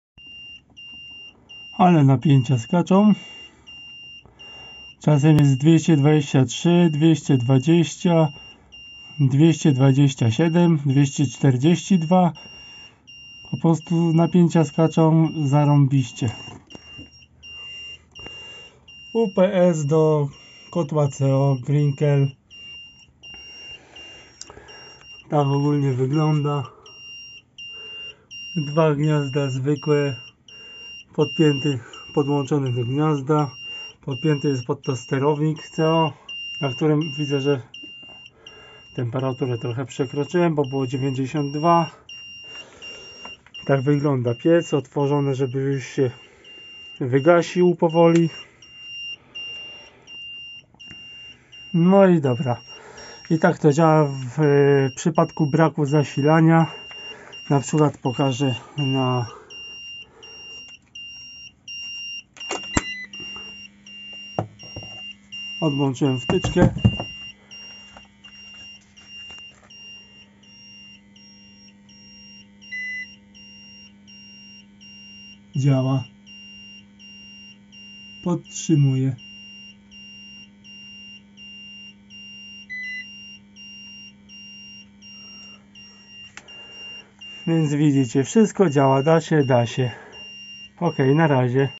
UPS w kotłowni
Ups wytrzymuje 3:00 godziny podtrzymania pracy pompy i dmuchawy i sterownika kotła c.o podczas blackout . Czyli przekładając na język polski braku możliwości dostarczania energii elektrycznej.